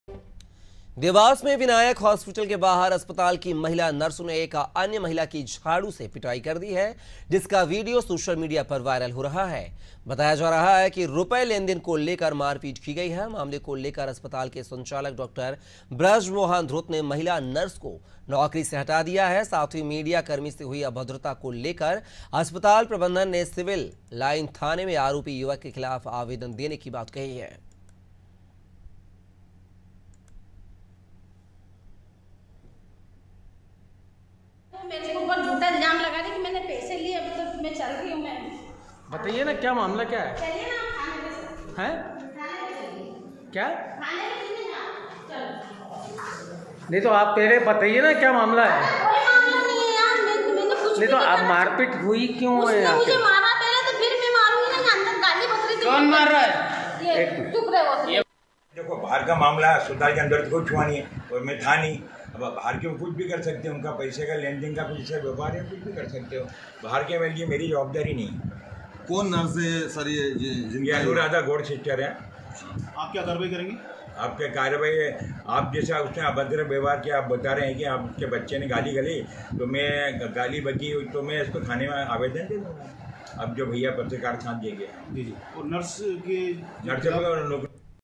Dewas : हॉस्पिटल के बाहर मारपीट, अस्पताल नर्स ने महिला को पीटा | Anaadi Tv
AIRTEL - 379